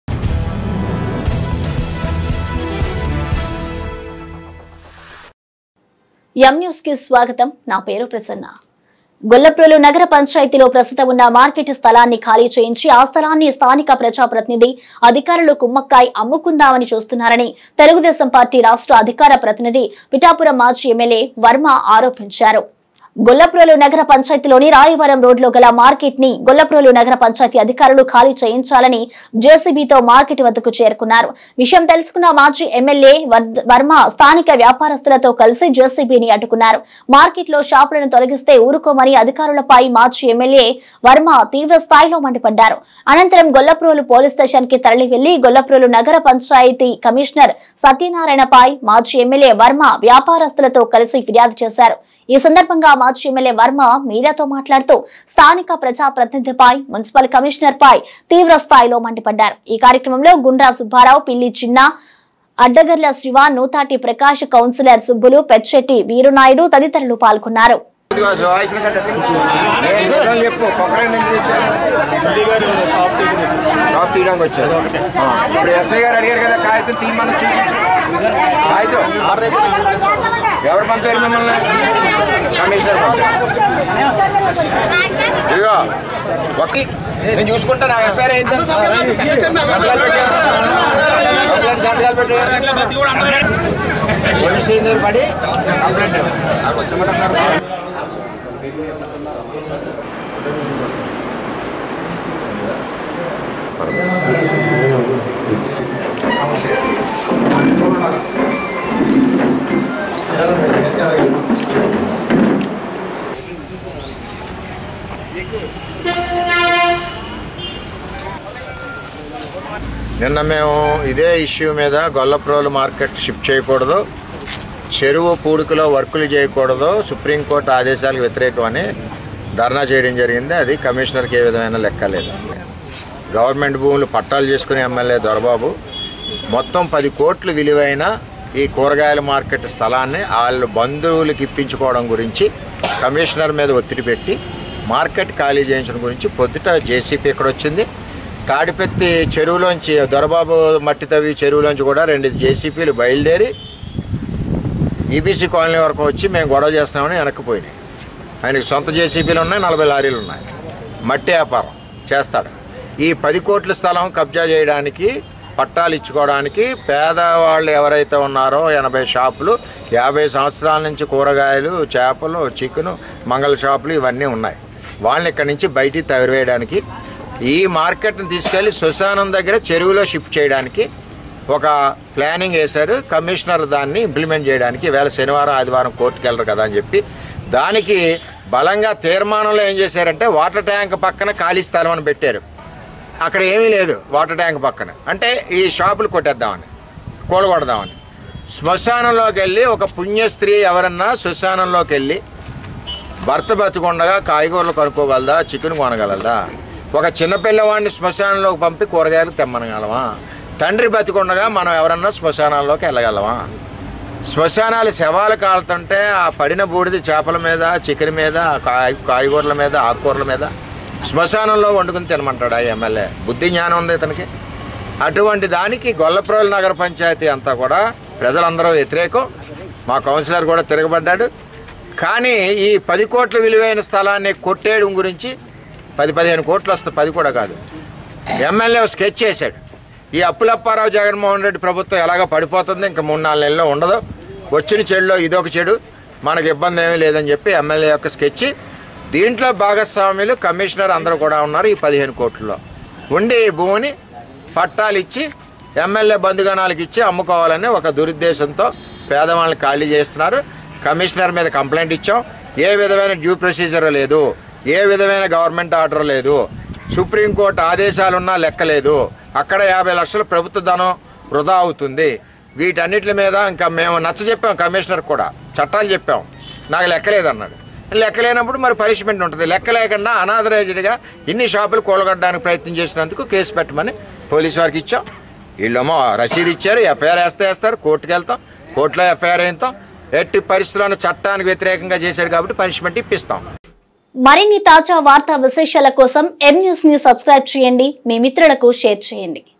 గొల్లప్రోలు నగర పంచాయతీలో ప్రస్తుతం ఉన్న మార్కెట్ స్థలాన్ని ఖాళీ చేయించి||M NEWS TELUGU
గొల్లప్రోలు నగర పంచాయతీలో ప్రస్తుతం ఉన్న  మార్కెట్ స్థలాన్ని ఖాళీ చేయించి,ఆ స్థలాన్ని స్థానిక ప్రజాప్రతినిధి,అధికారులు కుమ్మక్కై అమ్ముకుందామని చూస్తున్నారని తెలుగుదేశంపార్టీ రాష్ట్ర అధికార ప్రతినిధి,పిఠాపురం మాజీ ఎమ్మెల్యే వర్మ ఆరోపించారు.||M NEWS TELUGU